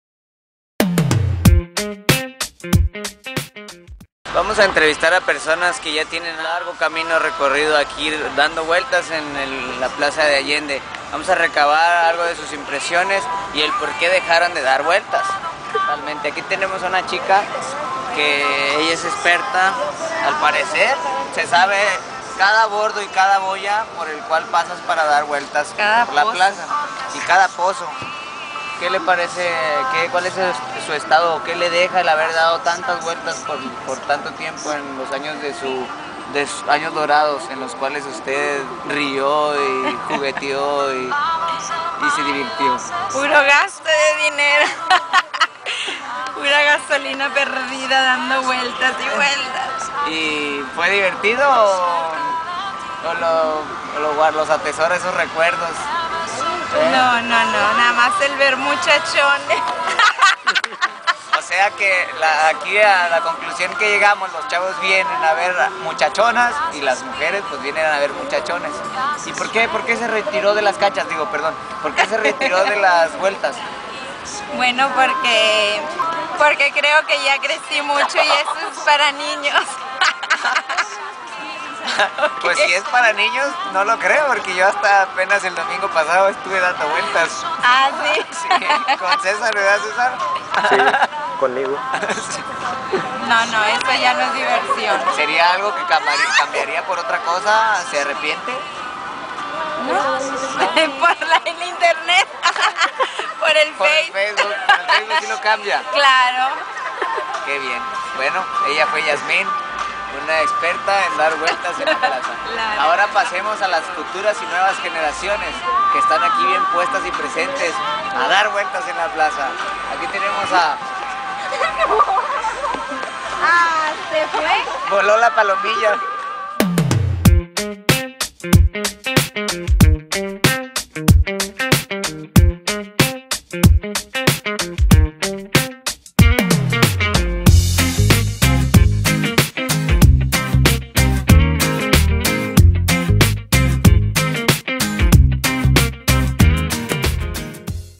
LIGACION EN LA PLAZA DE ALLENDE NUEVO LEON
Ir a la plaza los domingos por la noche es una costumbre de los Allendenses